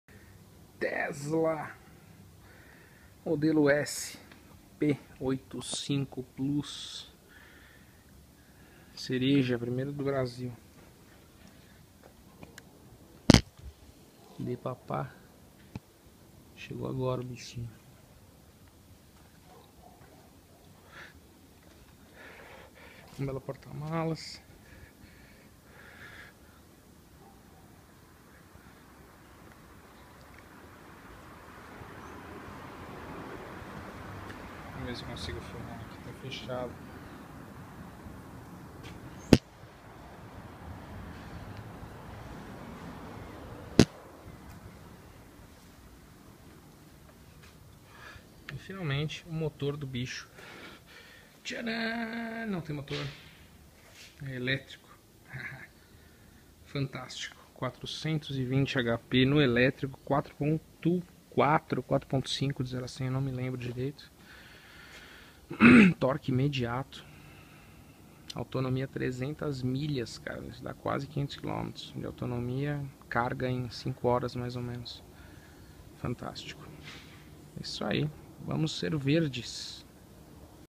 Tesla Model S P85+ Primeiro do Brasil
Com um motor 100% Elétrico de 420 hp com autonomia de 300 milhas